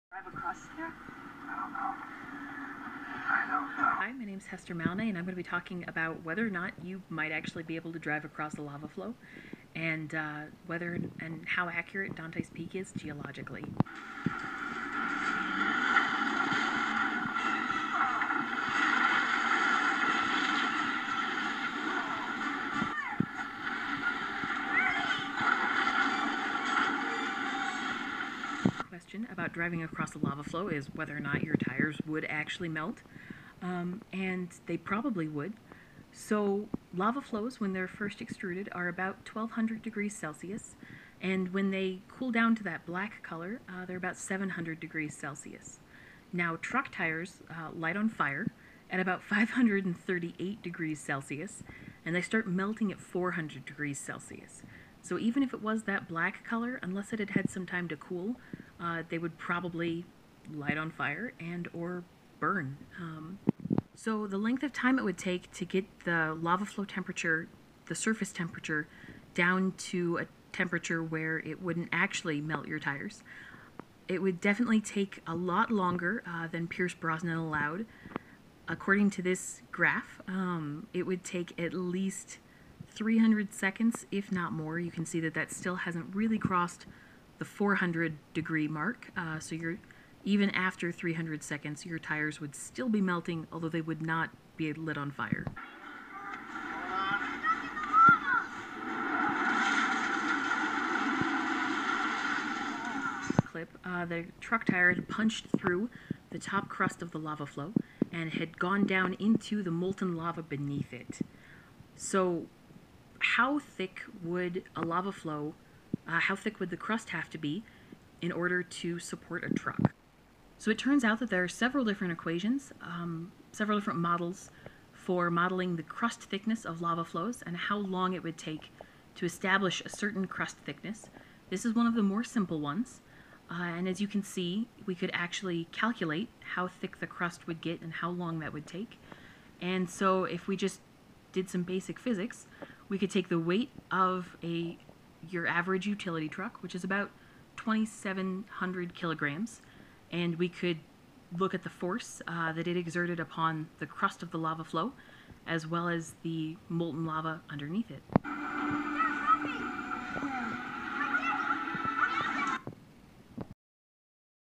Dante's Peak Discussion
Can you drive over lava?
(Video for Geology in Movies, Idaho State University, Spring 2017)